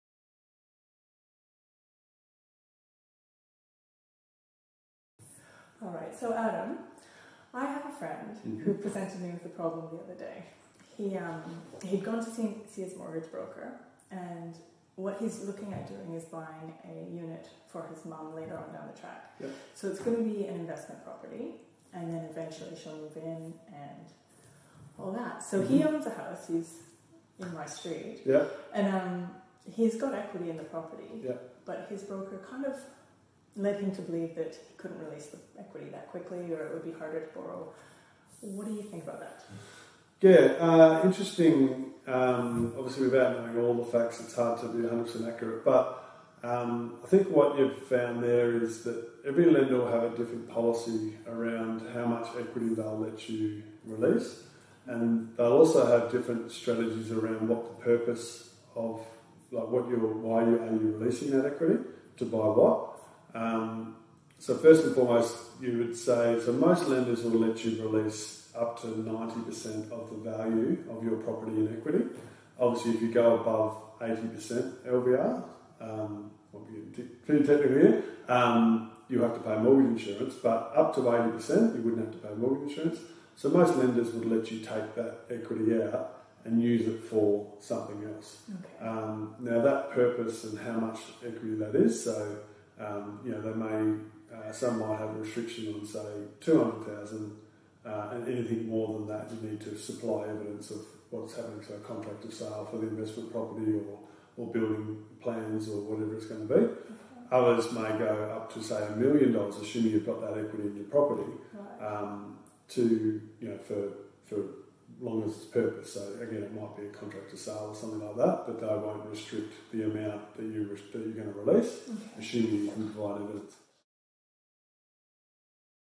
Dwell Building Services presents Adam from Loan Market - The Role of Purpose When Releasing Equity
In this conversation, Adam and Cassandra and discussing the role of Purpose in getting a loan. When you want to release equity from your current home, the purpose you want to apply that money to plays a big part.
If you're buying a home or looking at renovating, go and make contact!